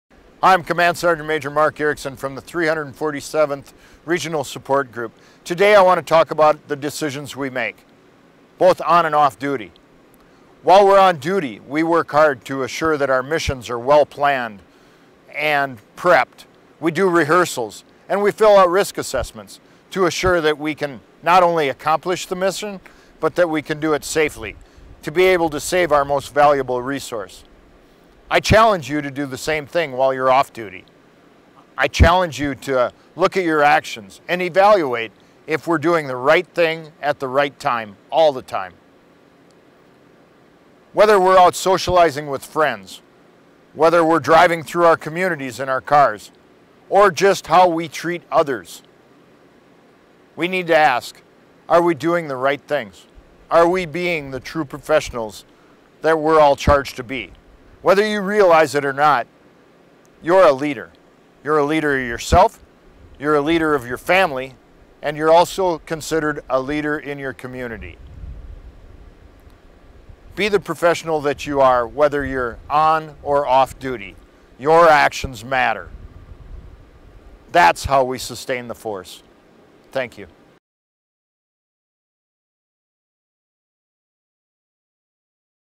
Brigade CSM - Your Actions Matter
Command Sgt. Maj. Marcus Erickson, 347th Regional Support Group command sergeant major,  wants to let Soldiers know that "Actions Matter," challenging each and every one of us to represent ourselves well both on and off duty.